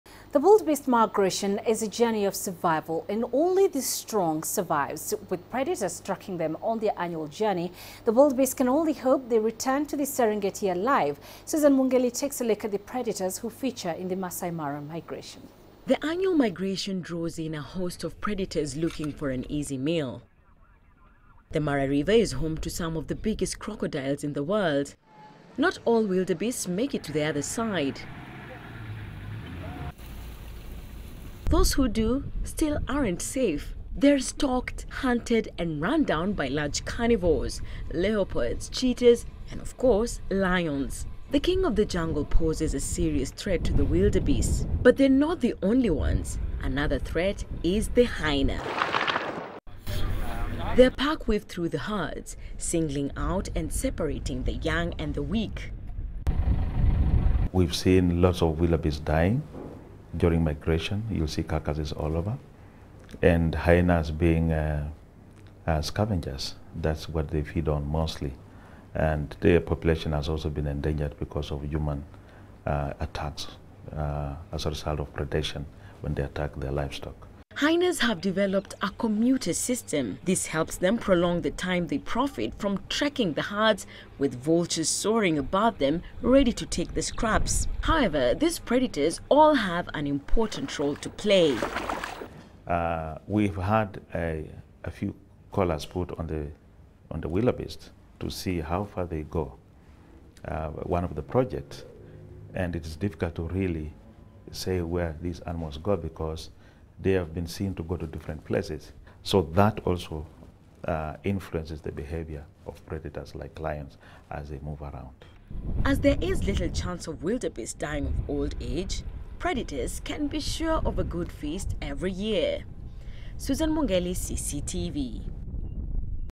The role of the predators in the wildebeest journey
The wildebeest migration is a journey of survival -- and only the strong survives. With predators tracking them on their annual journey, the wildebeest can only hope they return to the Serengeti alive. Susan Mwongeli takes a look at the predators who feature in the Maasai Mara migration.